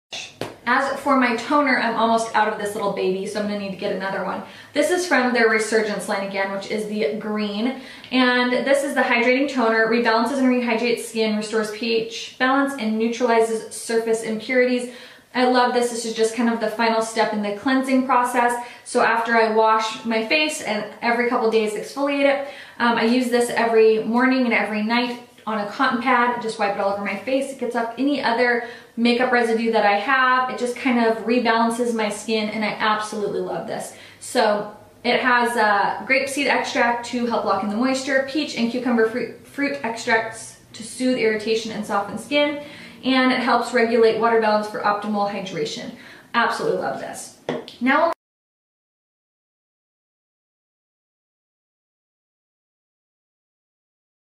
Review Toner hồi sinh và thêm ẩm Murad Hydrating giúp trẻ hóa da
Murad Hydrating Toner với thành phần Lecithin sẽ giúp khóa nước trên làn da và phục hồi sự đầy đặn của da. Đây là phương pháp tốt nhất để chuẩn bị cho các bước chăm sóc và điều trị da tiếp theo.